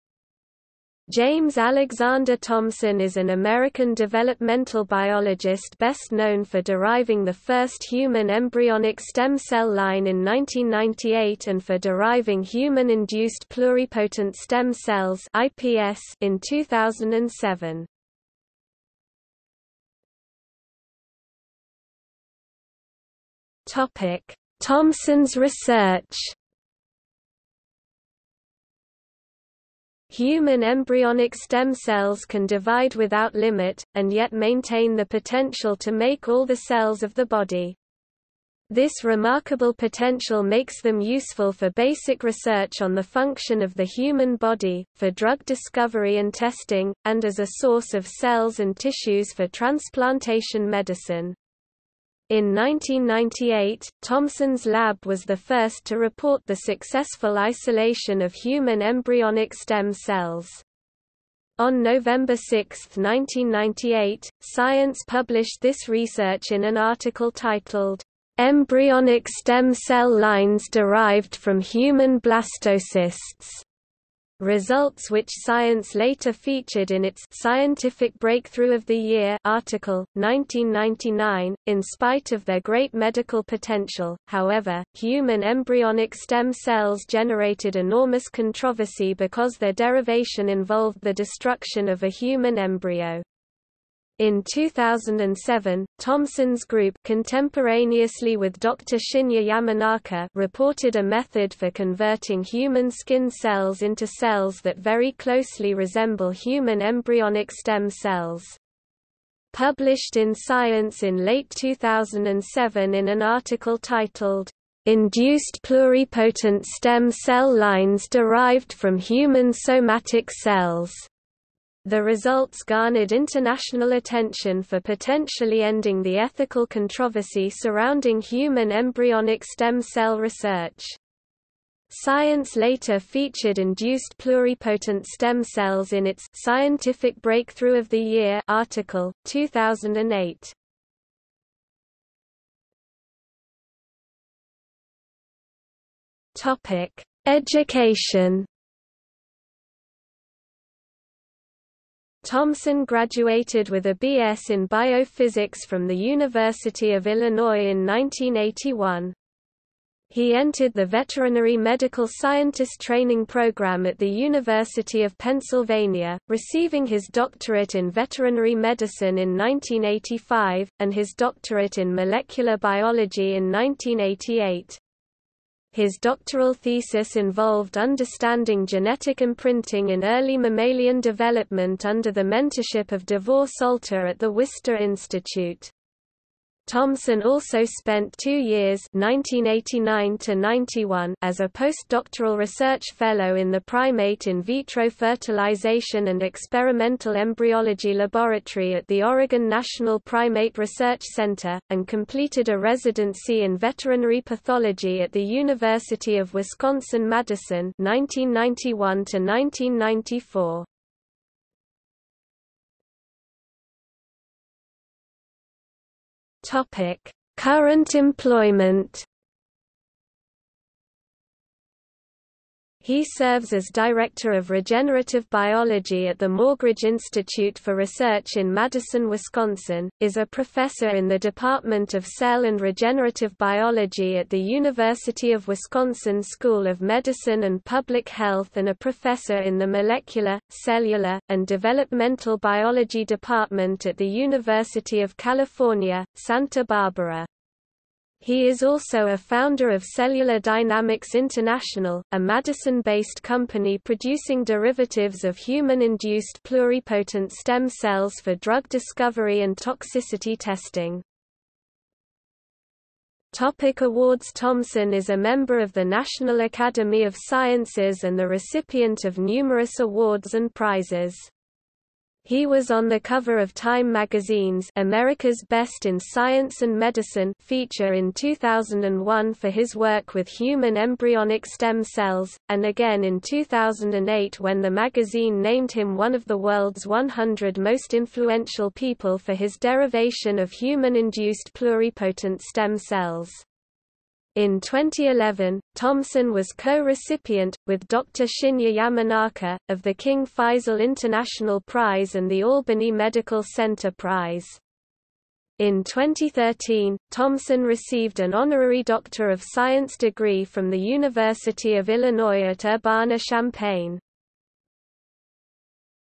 James Thomson (cell biologist) | Wikipedia audio article
00:00:20 1 Thomson's research
00:02:22 2 Education
00:03:32 3 Current employment
00:04:23 4 Awards

- Socrates

SUMMARY
James Alexander Thomson is an American developmental biologist best known for deriving the first human embryonic stem cell line in 1998 and for deriving human induced pluripotent stem cells (iPS) in 2007.